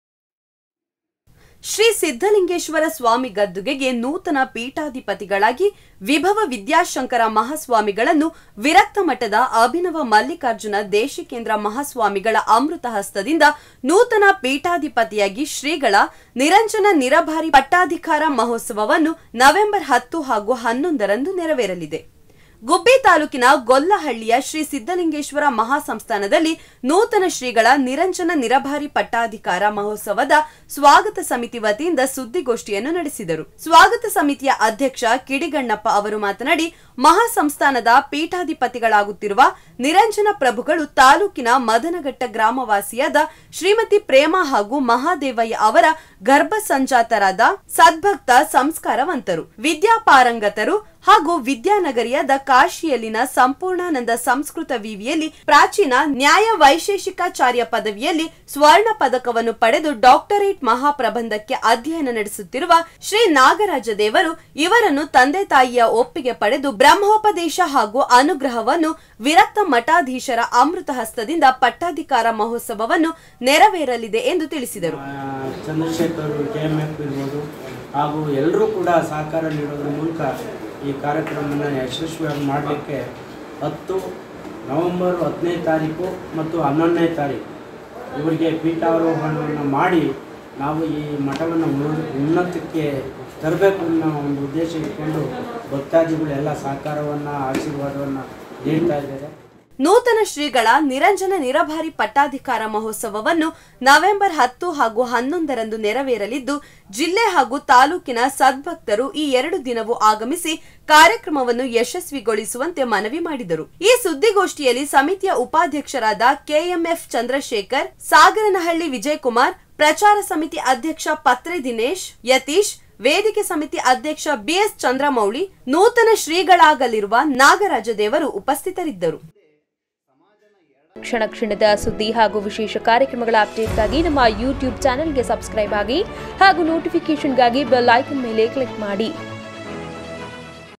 ಶ್ರೀ ಸಿದ್ದಲಿಂಗೇಶ್ವರ ಸ್ವಾಮಿ ಗದ್ದುಗೆಗೆ ಶ್ರೀ ನಾಗರಾಜದೇವರ ಪಟ್ಟಧಿಕಾರ
ಶ್ರೀ ಸಿದ್ದಲಿಂಗೇಶ್ವರ ಸ್ವಾಮಿ ಗದ್ದುಗೆಗೆ ನೂತನ ಪೀಠಾಧಿಪತಿಗಳಾಗಿ, ಶ್ರೀಗಳ ನಿರಂಜನ ನಿರಭಾರಿ ಪಟ್ಟಧಿಕಾರ ಮಹೋತ್ಸವವನ್ನು ನಡೆಸಲು ಸ್ವಾಗತ ಸಮಿತಿ ವತಿಯಿಂದ ಸುದ್ದಿಗೋಷ್ಠಿಯನ್ನು ನಡೆಸಿದರು.